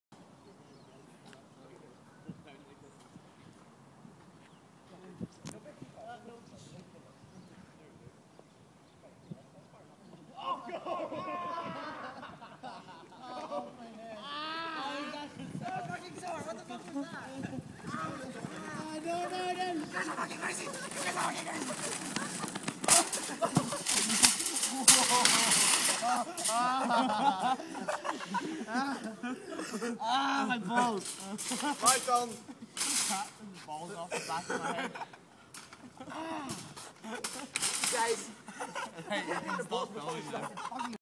Trolley stunt
Us doing jackass trolley stunt in ayr